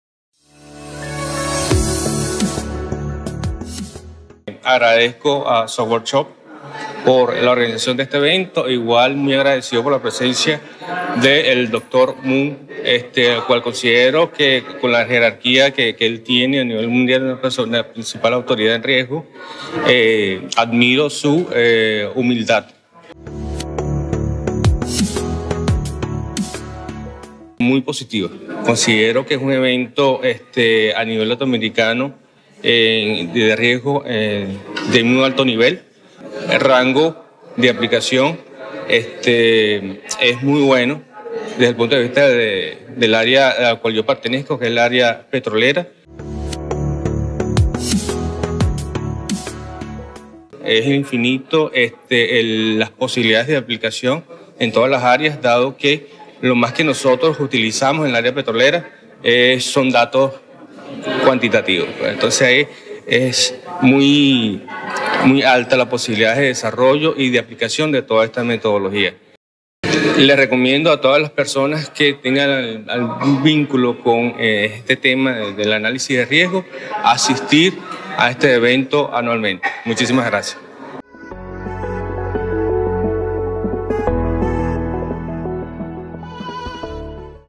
SoyCQRM - José Ramírez
Testimonios de expositores y participantes en la Convención Latinoamericana en Métodos Cuantitativos y Gestión de Riesgo.

Entrevista a José Ramírez, Asesor de Información de Estadística en PDVSA Venezuela con certificación CQRM